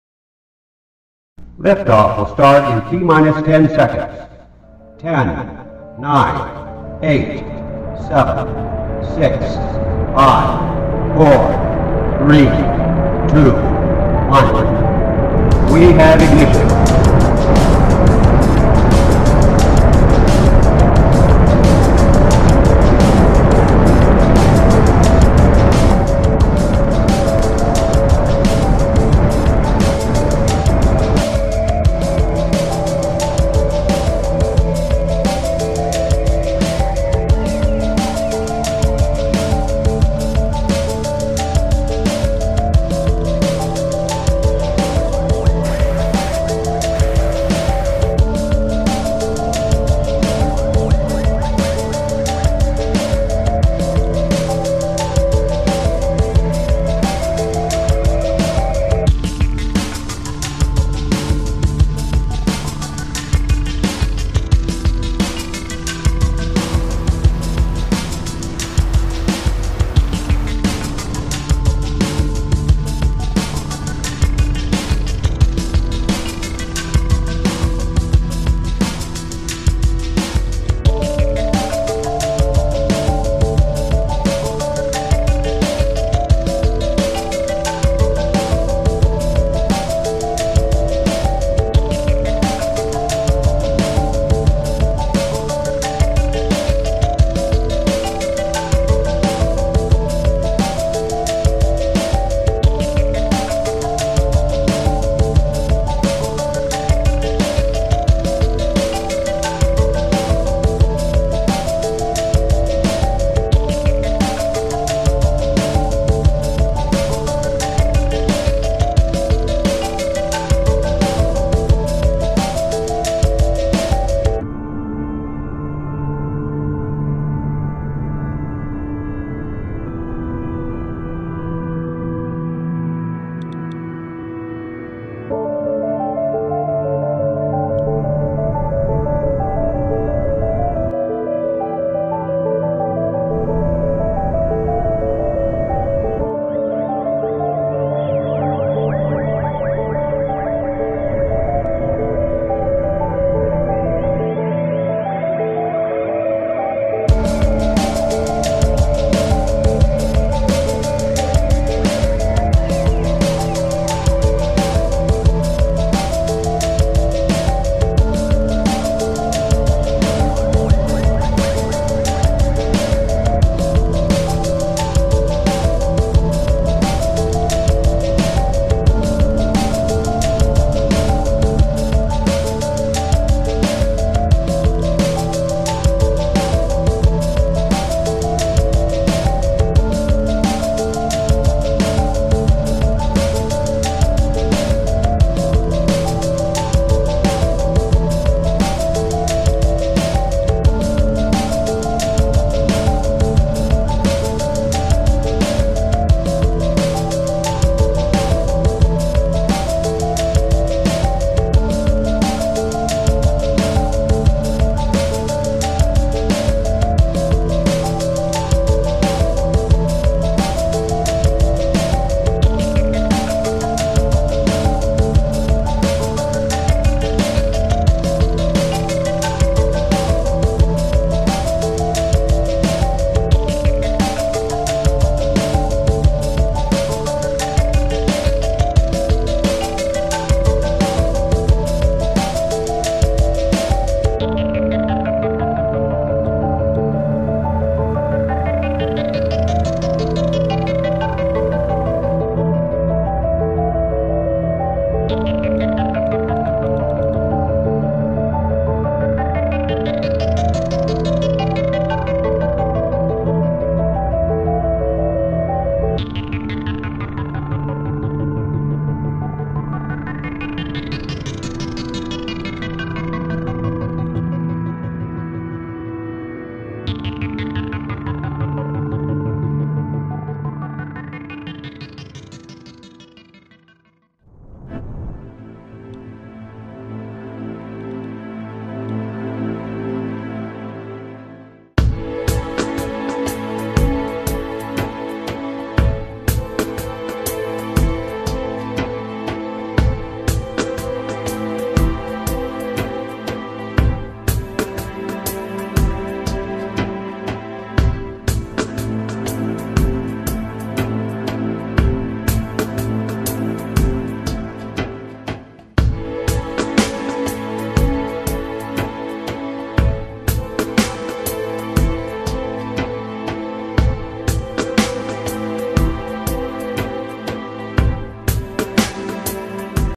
Journey to SATURN | SFS short movie
Hi everyone and welcome again. I've been editing this for many days, wich took several hours to record and edit. I really hope you like it, so let me know it un the comments!

We're visiting Enceladus and Titan and coming back home to recover science data from our expedition.

Graphic resourses: Cassini-Huygens multimedia